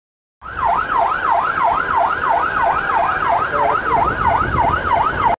CCSS realizará traslado convoy de pacientes covid de San Carlos
• Seis pacientes en condición grave serán trasladados desde el hospital San Carlos a tres centros médicos capitalinos.

• Tres equipos Primera Intervención Médica Especializada (PRIME) se desplazan en este momento para traer pacientes junto con tres Agrupaciones Regionales de Traslado Especial (ARTE) del hospital San Carlos.